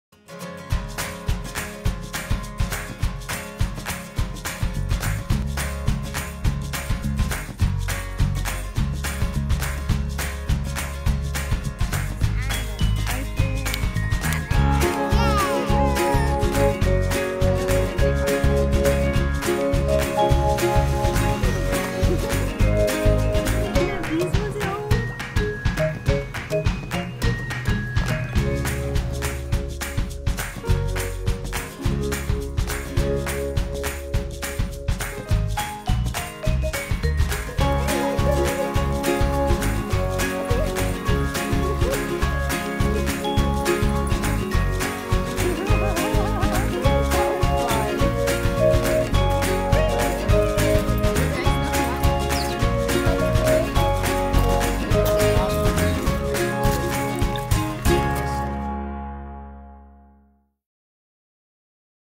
Byodo In Temple with Kids On Air
Kids On Air Hawaii's Educational Day Program - Visiting the Buddhist Temple in Kaneohe.